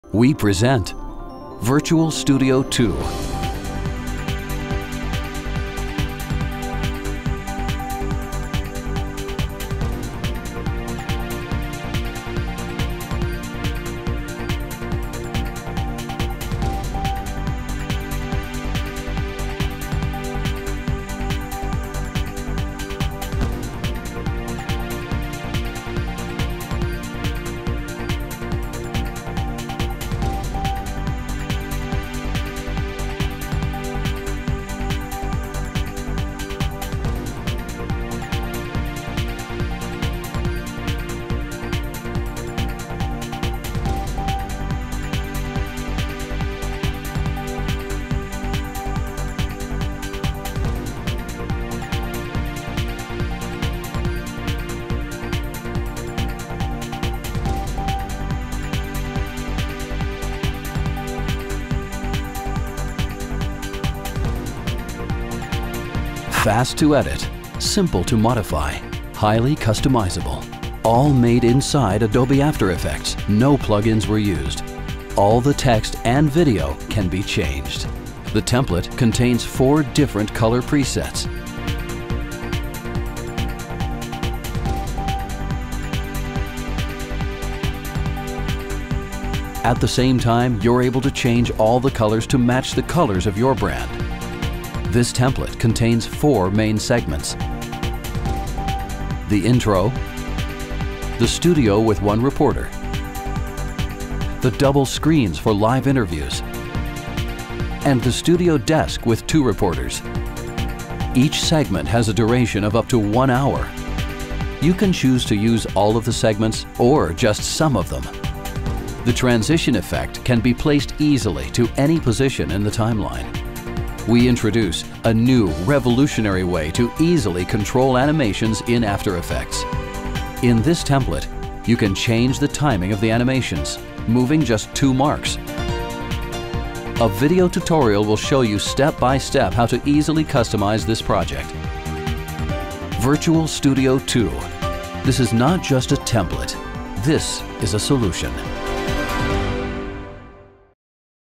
Virtual Studio 2 After Effects Template
We have virtual studio backgrounds in Quicktime and Adobe After Effects formats.

 Description:
Virtual Studio 2 is the template that was created for editors who want to incorporate a green screen video with a presenter in there projects.

This project was built to be extremly easy to edit, wile being highly versatile at the same time. This way editors can use it for a large variety of projects.

The main features: - The duration can be changed easily to fit the exact duration of your presentation. All the scenes and sub-compositions can have a duration up to 1 hour

- You can edit the timing of all the animations by moving just two marks. This is a revolutionary new way to construct projects, so that you can change timing and animation very easily. You will love this new feature!

- We have included 4 colors setups, at the same time the template has a special option allowing you to change ALL THE COLORS to mach the colors of your brand.

- A detailed Video Tutorial is included to show you step by step how to edit the project.

- The template has 4 main segments:
1. Intro
2. Studio whit one reporter
3. double screen for live interviews
4. studio desk with two reporters

These segments are made in such a way that you can use any of them, and create cuts between them easily. The transition effect can be placed over the cuts whit just 2 clicks.

Made all in After Effects, NO plug-ins ,NO prerenders used you can change everything in this project using only After Effects.